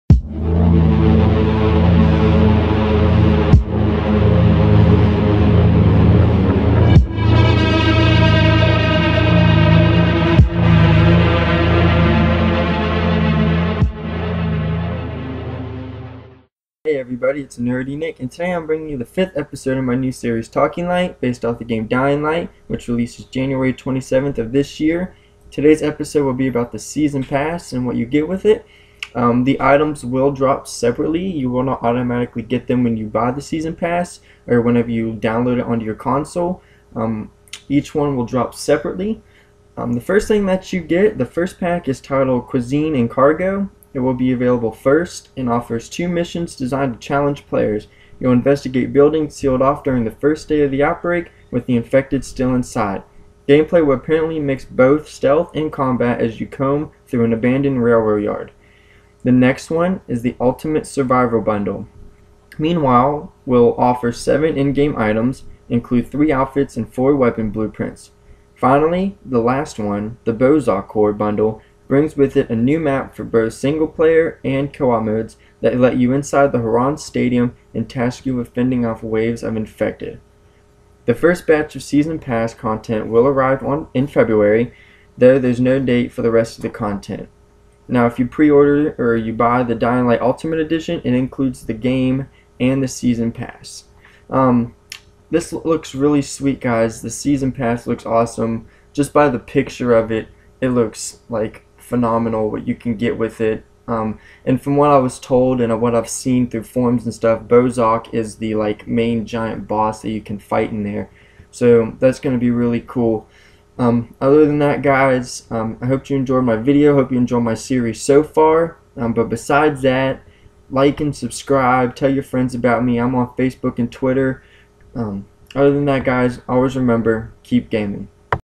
Talking Light: Season 1 Episode 5: Season Pass
Fifth episode of my new series Talking Light based off the game Dying Light. Picture courtesy of Gamestop. This is about what the season pass has to offer a brief description of what to expect from each season pass item. Hope you guys like my new series!!! Sorry I don't have my own footage of the game yet. As soon as it comes out I will!!!